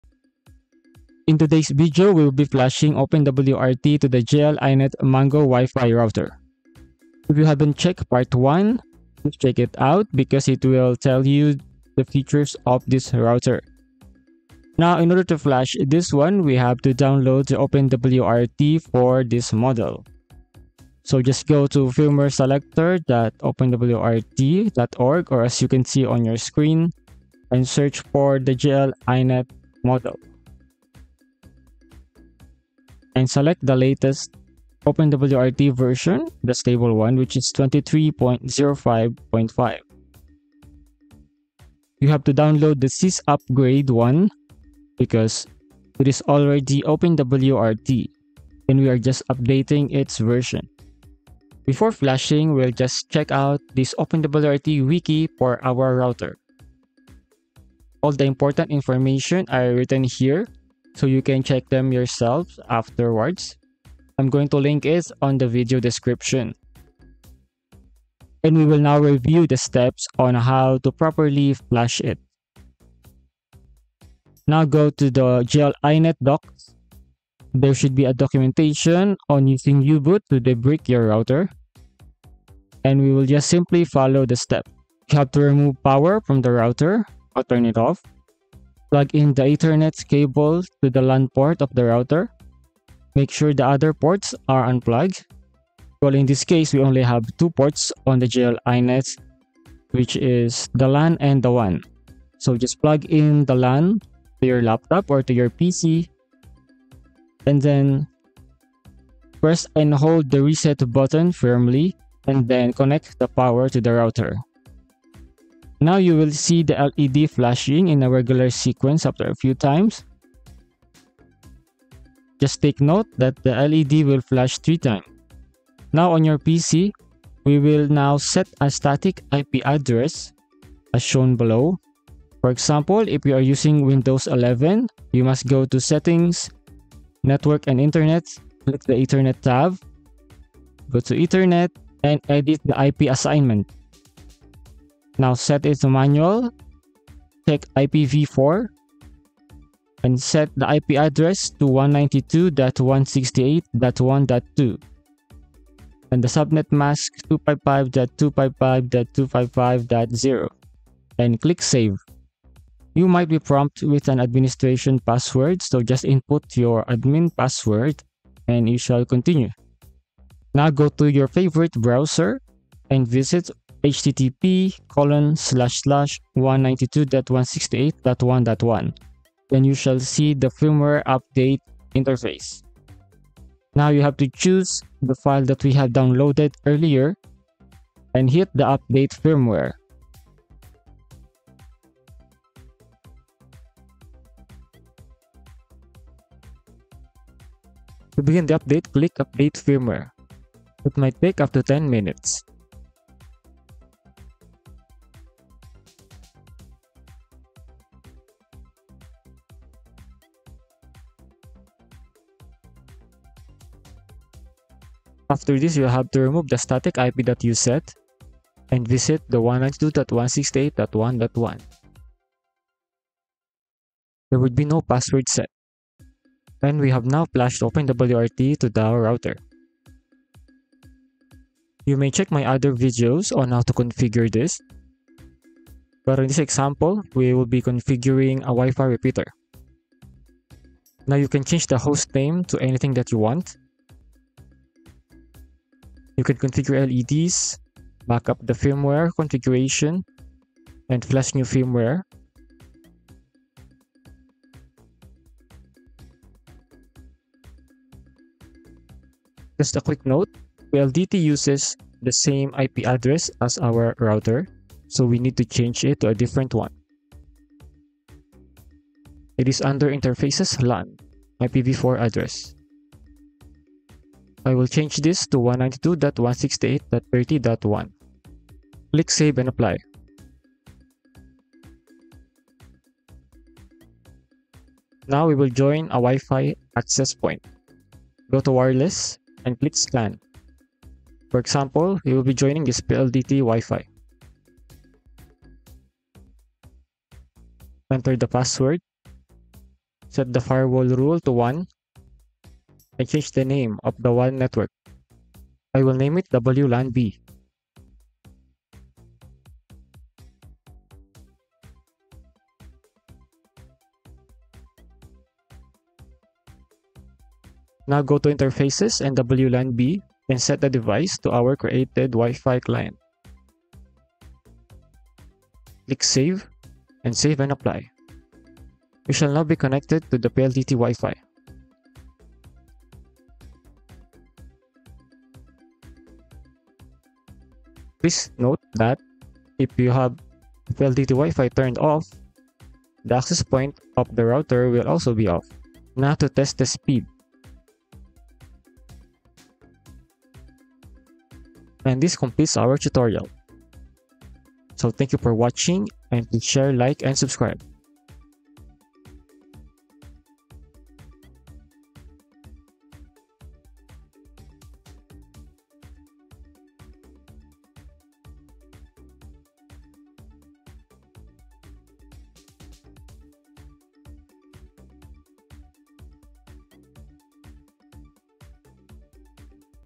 Flashing OpenWRT to Mini Travel Router
Short video on how to flash OpenWRT to mini travel router (GL Inet MT300N-V2) with WiFi Repeater Demo.